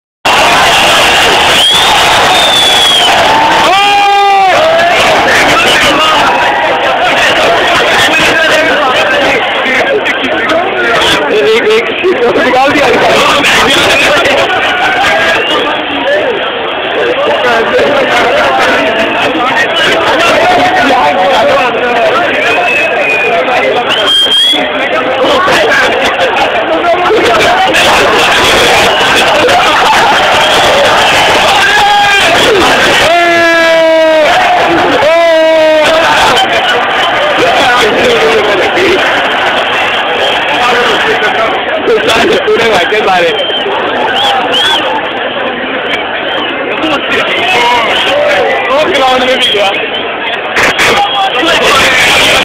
Kotla ka Ghot(a)la
27th dec 2009.. india vs sri lanka 5th odi The final one-dayer between India and Sri Lanka was called off due to unfit playing conditions at the Ferozeshah Kotla ground. WHO IS RESPONSIBLE??? ICC, BCCI, DDCA, PITCH COMMITTEE etc.